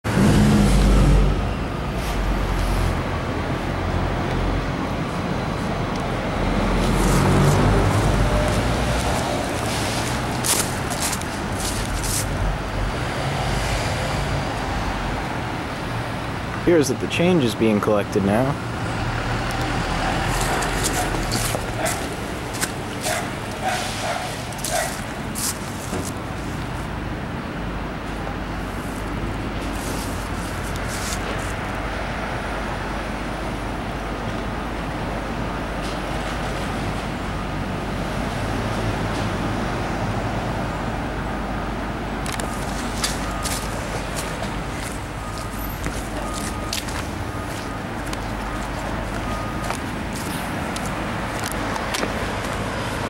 2014 11 06 AKPF Empties Meters of Change
Robin Hooding and chalking in downtown Keene, NH. Early morning AKPF sighting, agent empties change from meter